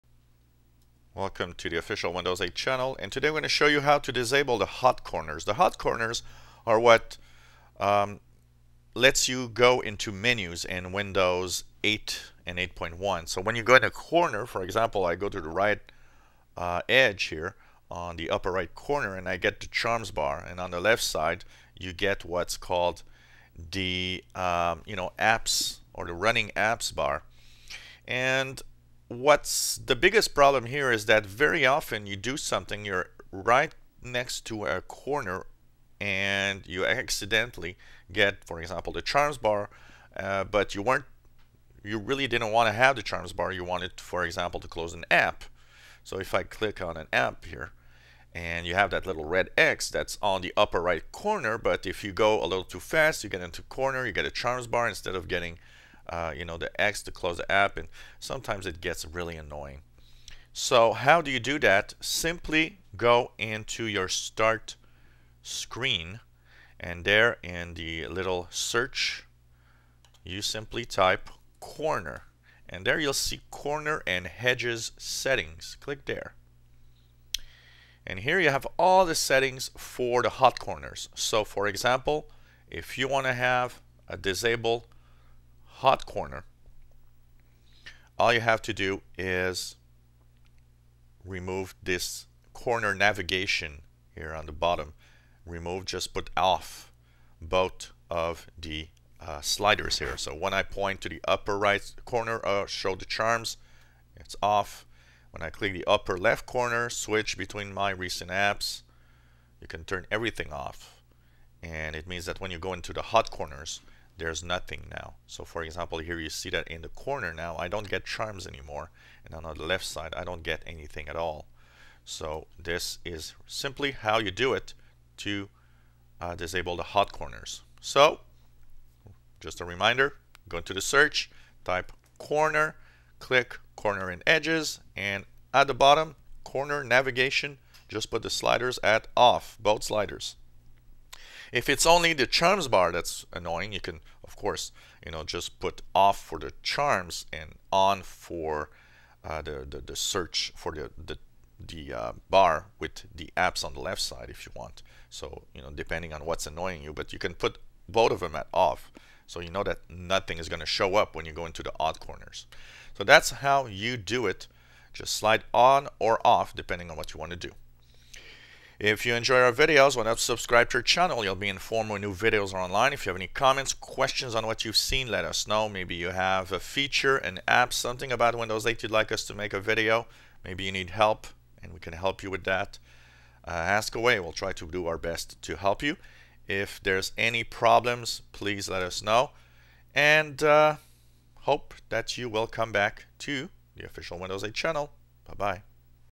Windows 8.1 How to disable the hot corners for charms and apps
How to disable annoying hot corners in Windows 8.1